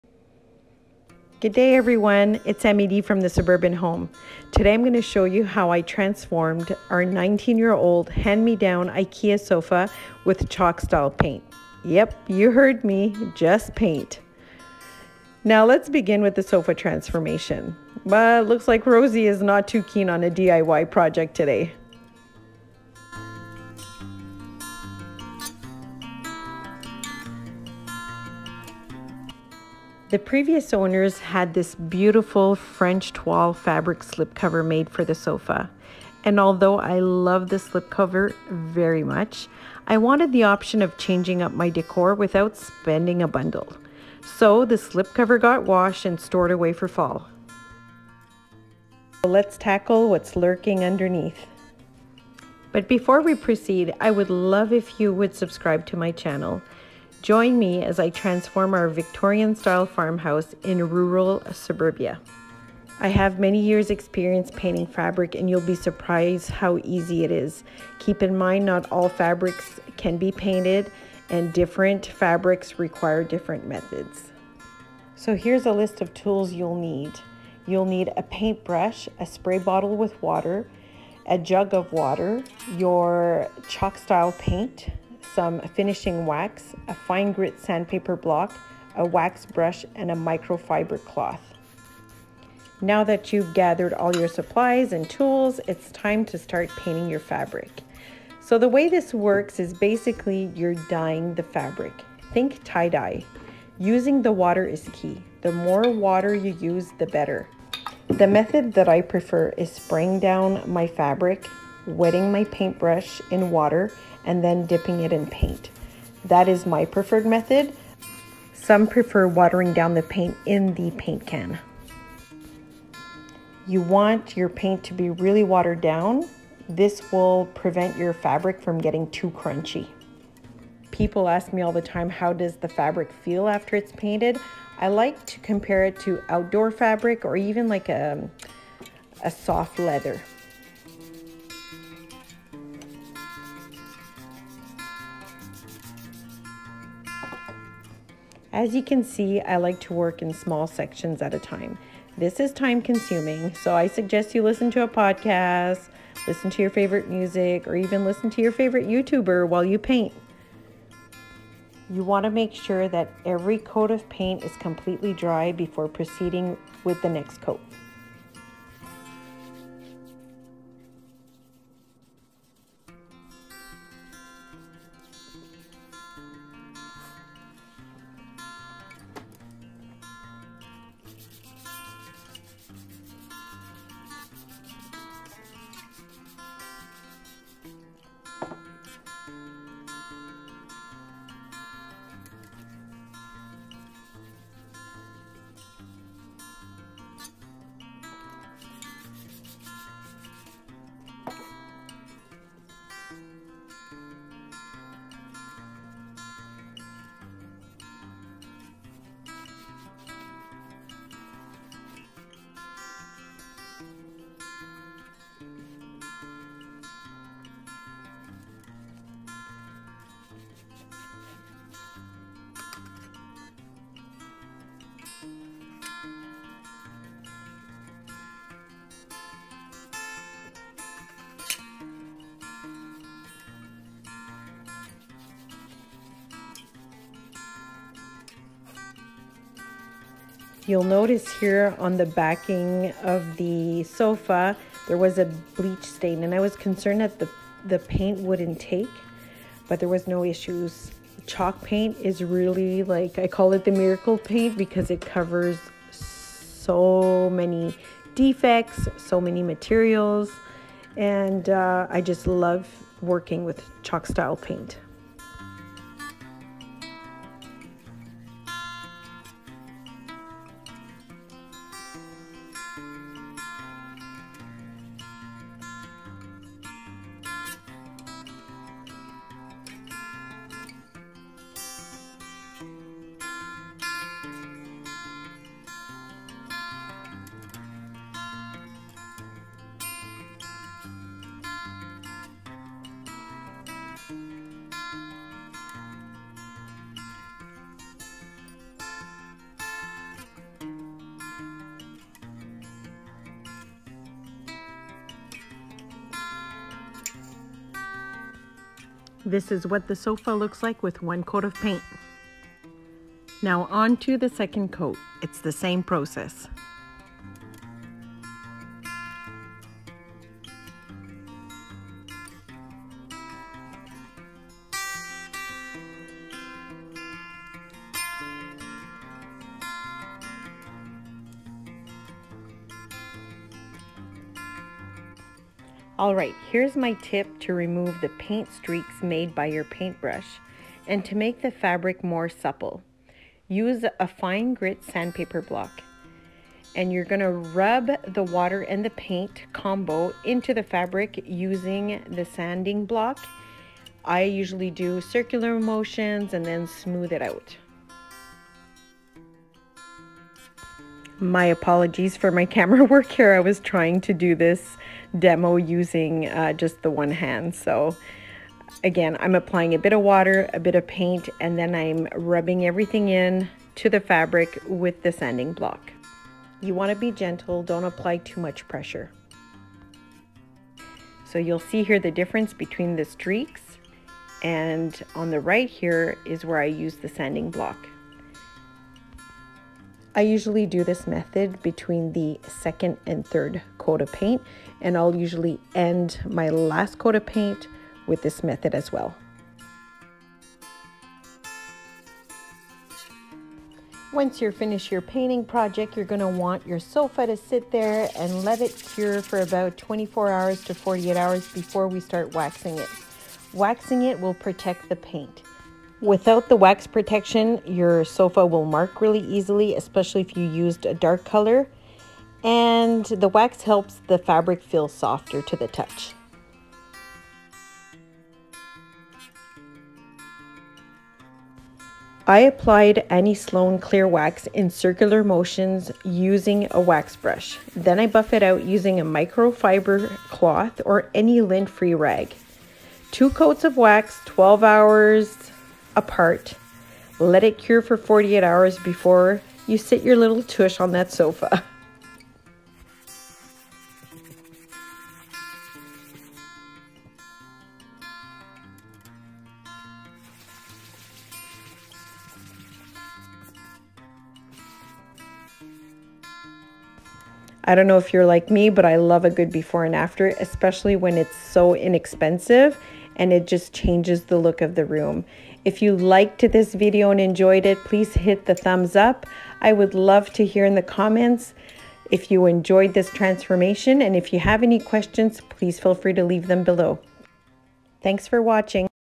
Paint that old sofa! | Sofa makeover | Chalk Paint Fabric
Join me as I transform our old hand me down IKEA ektorp sofa with some chalk style paint and a little elbow grease.